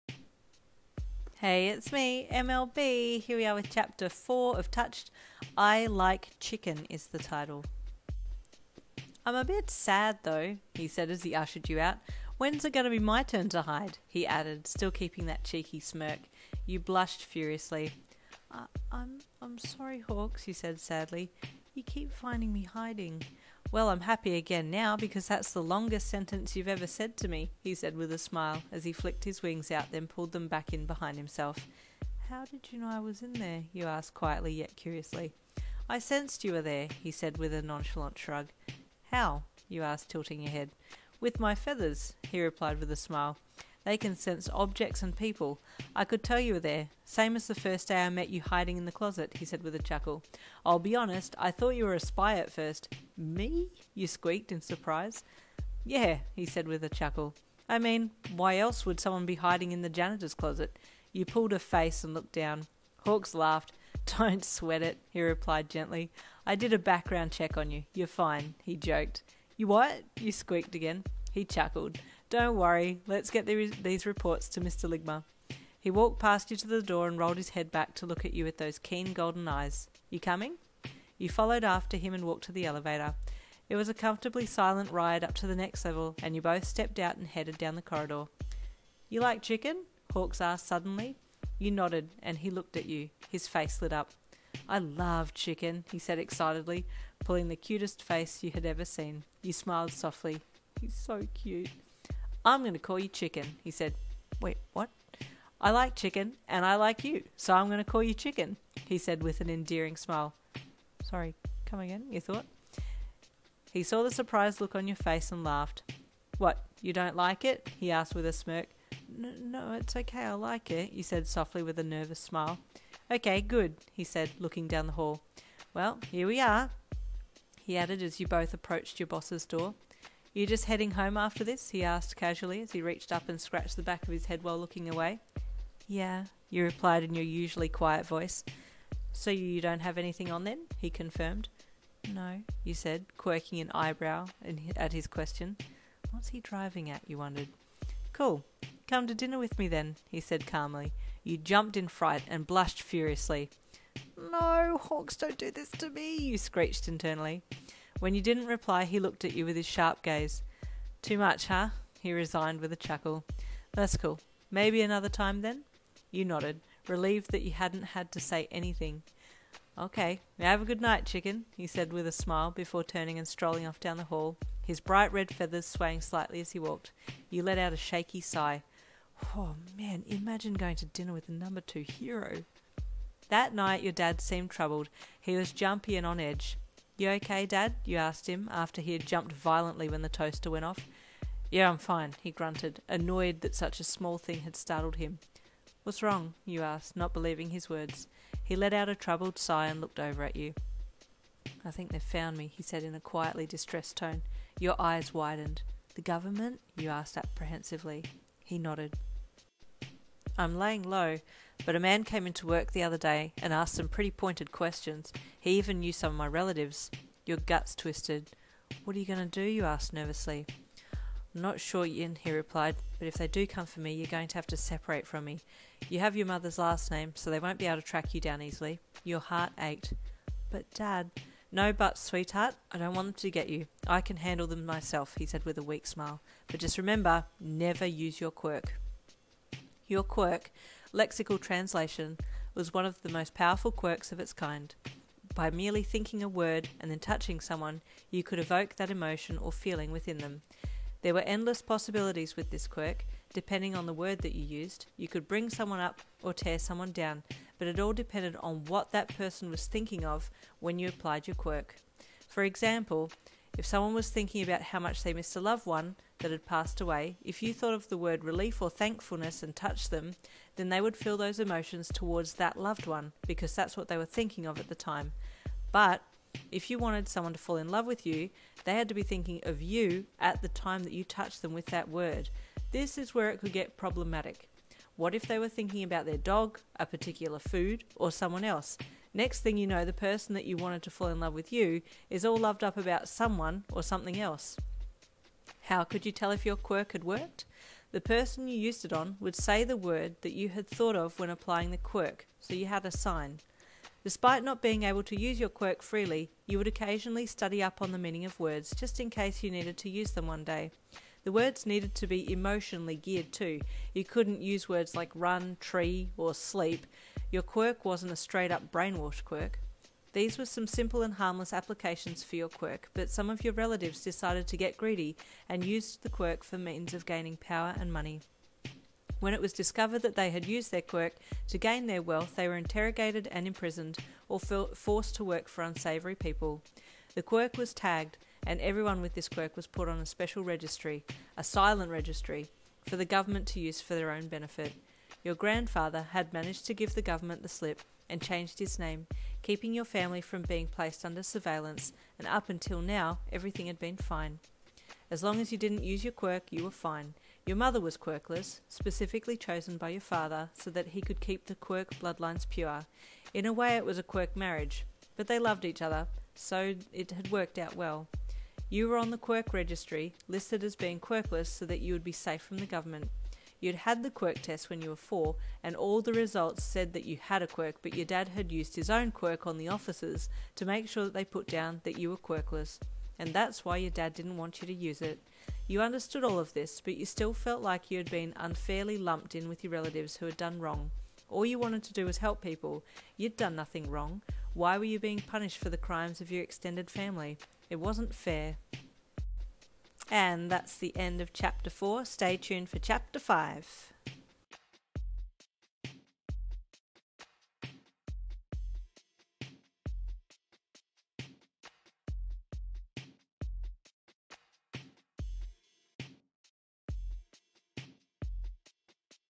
Touched - Hawks x Female Listener Chapter 4 Fanfiction
The government is starting to close in on your dad, and you also get to find out what your quirk is!

Touched - Hawks x Female Listener (Chapter 4)

All credit for the character Keigo Takami (Hawks) goes to Kohei Horikoshi and his amazing work - Boku No Hero Academia.